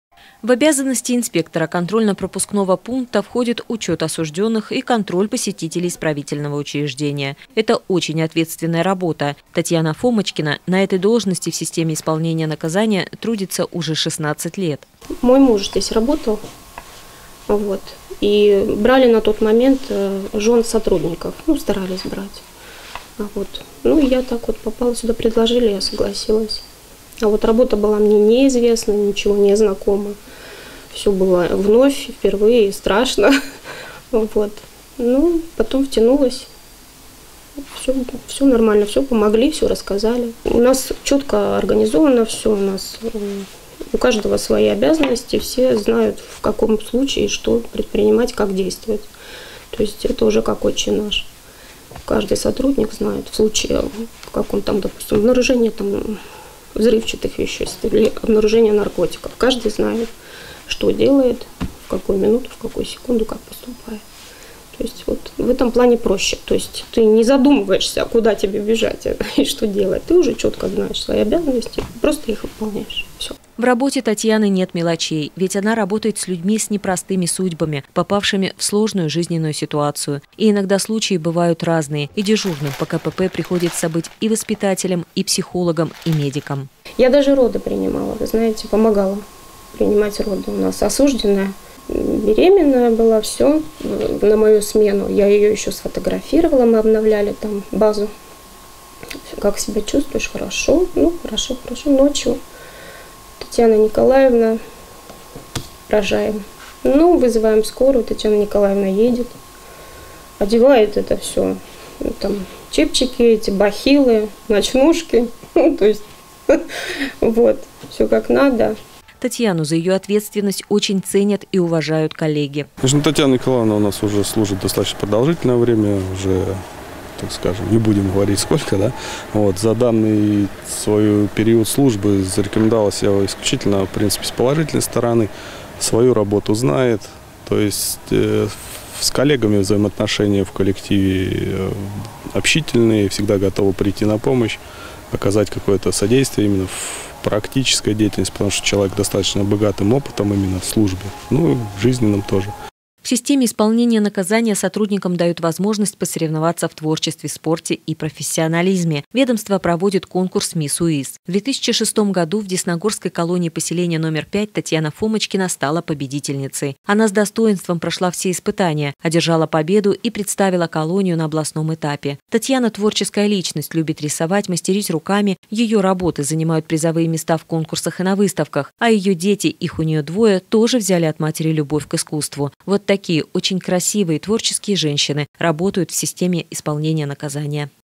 Десна-ТВ: Не женская профессия в исправительной колонии
В последние годы очень часто в системе исполнения наказания, должности, требующие особого внимания и ответственности, занимают женщины. В колонии-поселении №5 трудятся одиннадцать. В нашем следующем сюжете мы расскажем, кто ведет пропускной контроль на проходной этого исправительного учреждения.